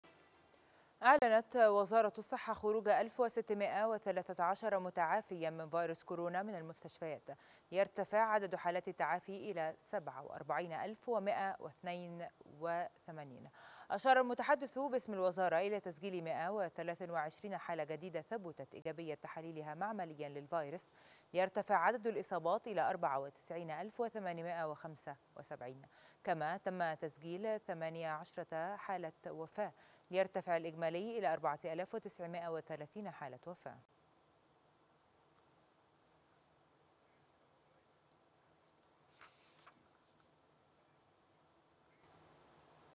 غرفة الأخبار | الصحة: تسجيل 123 حالة إصابة جديدة بالفيروس و18 حالة وفاة
5 أغسطس 2020